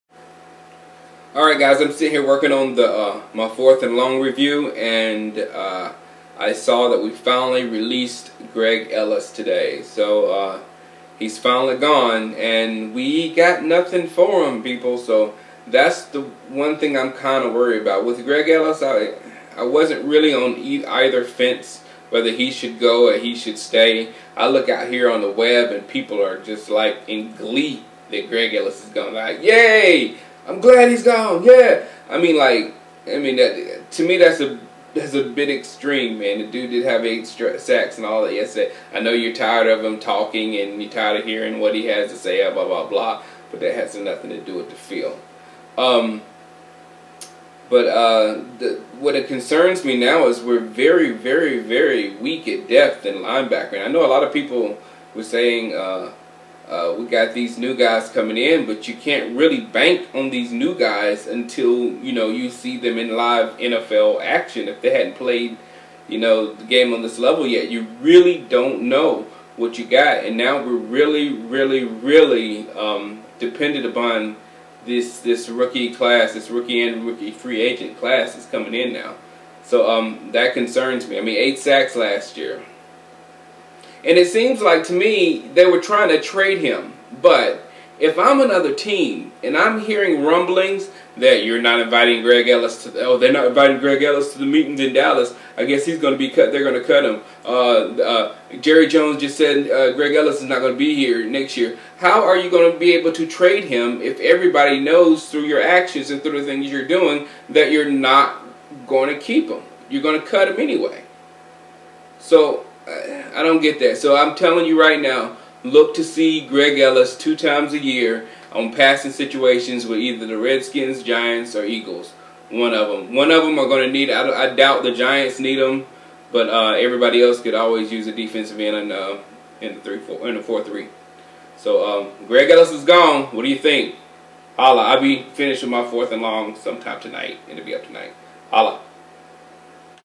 Cowboys officially cut Greg Ellis
After weeks of the most unkept secret in Valley Ranch..Greg Ellis is no longer a member of the Dallas Cowboys